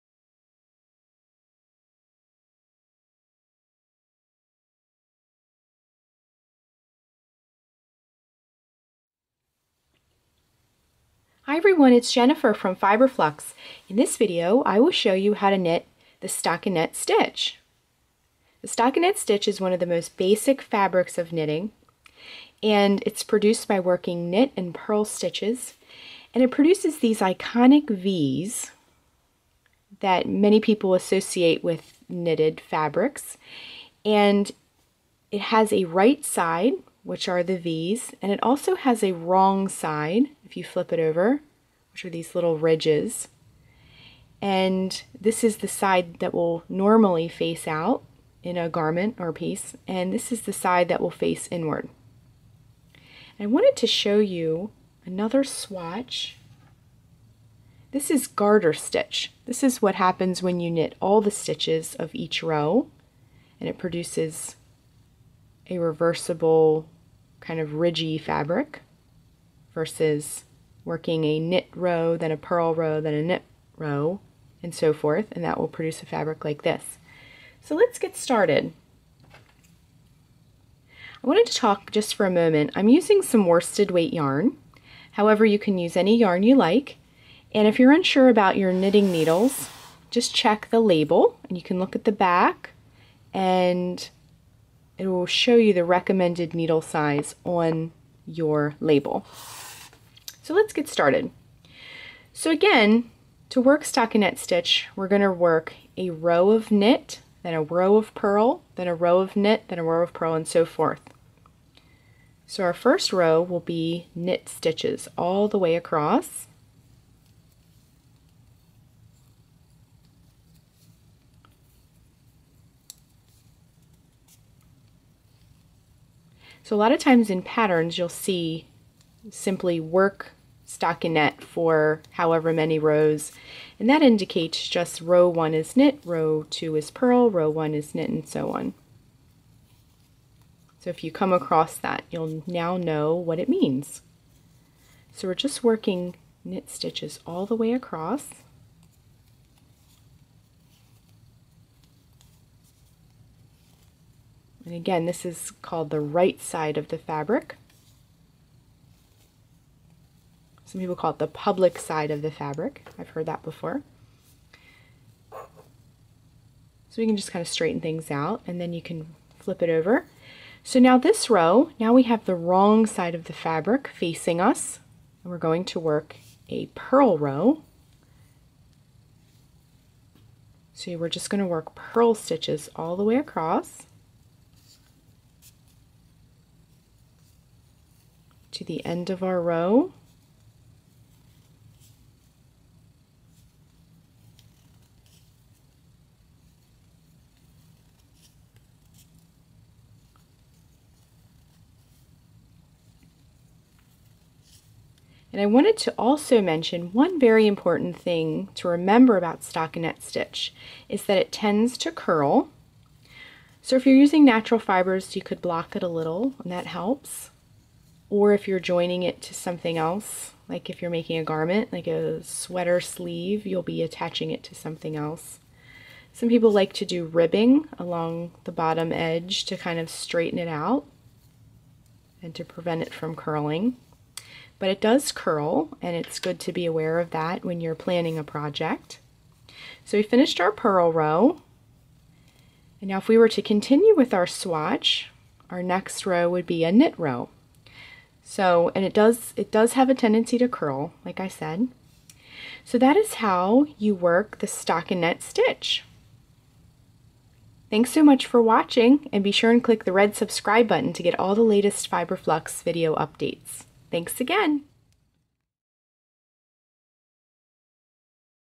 Episode 104: How to Knit the Stockinette Stitch
Learn how to knit the stockinette stitch with this easy tutorial!